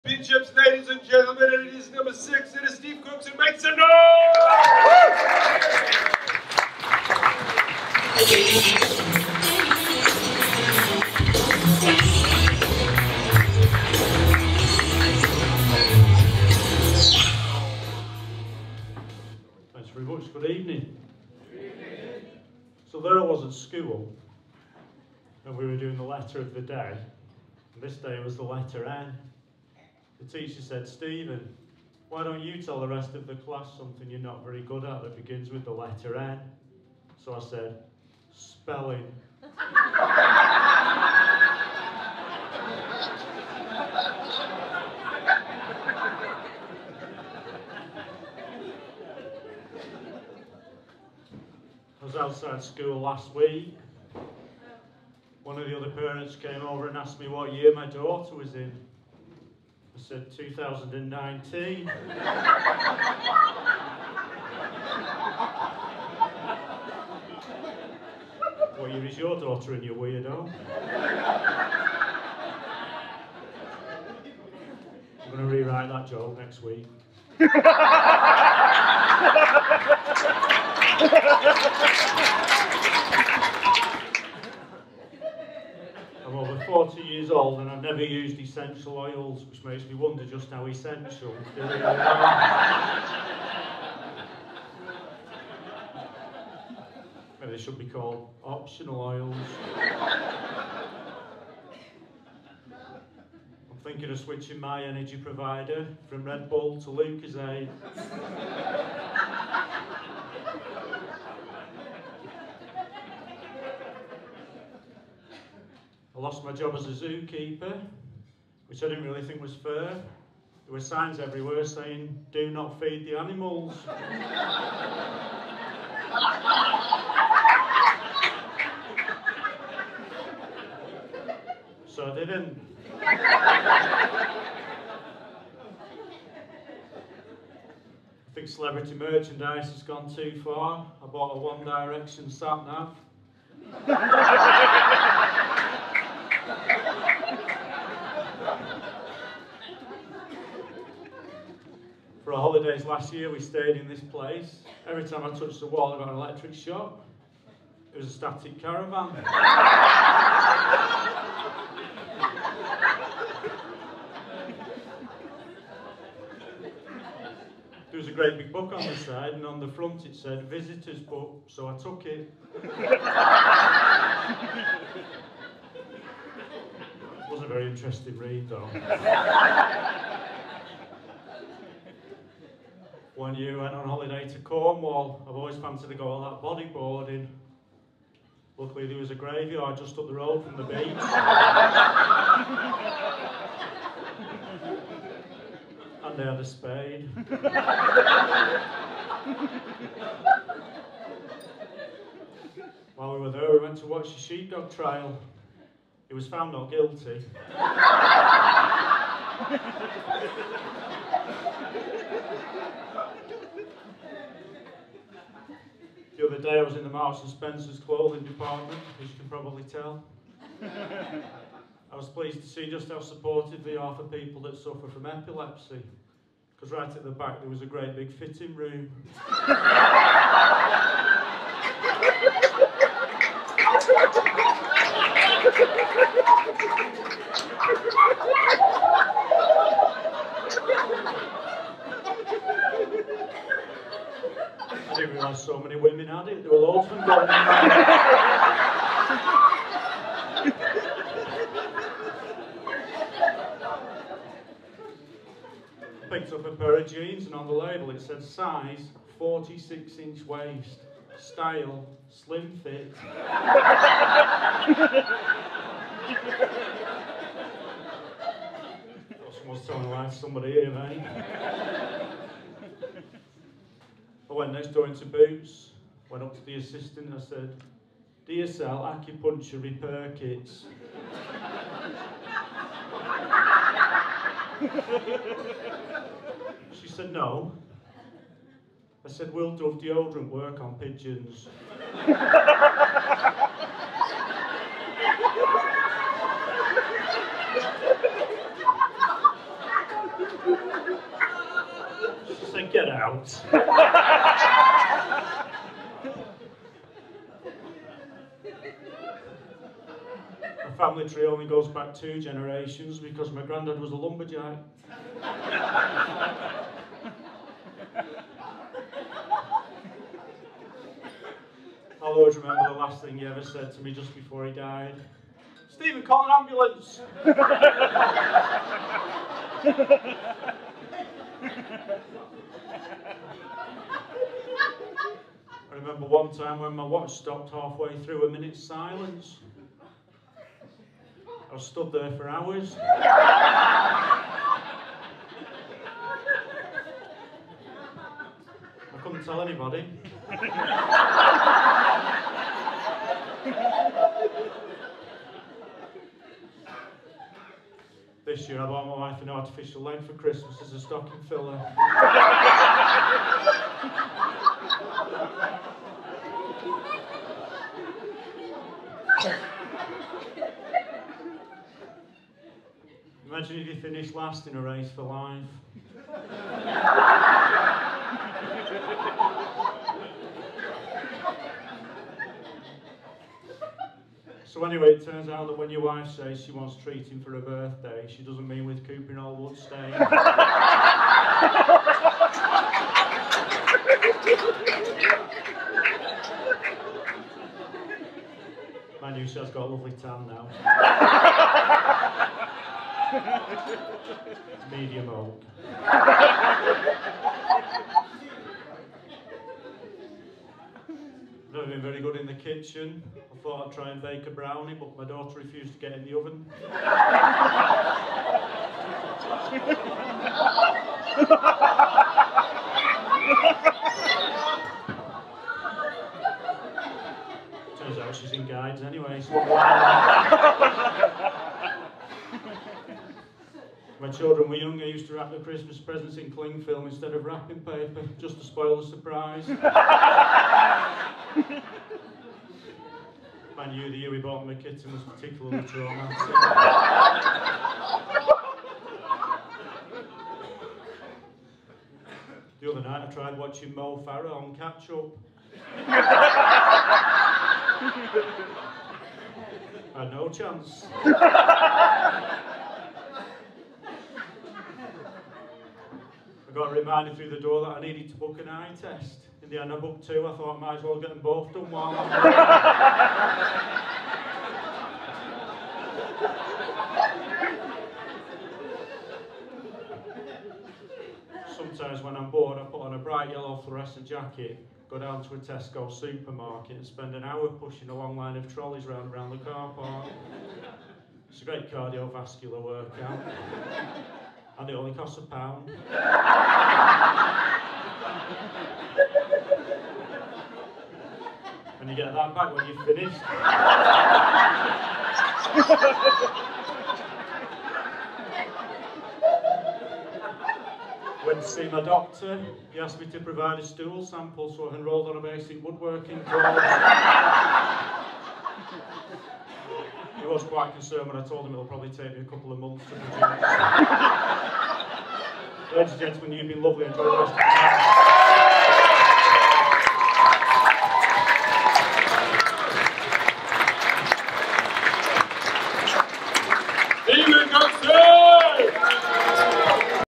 Steve Cookson at ROFL Comedy Club
Steve Cooksons winning entry into the World One Liner Championships at ROFL Comedy Club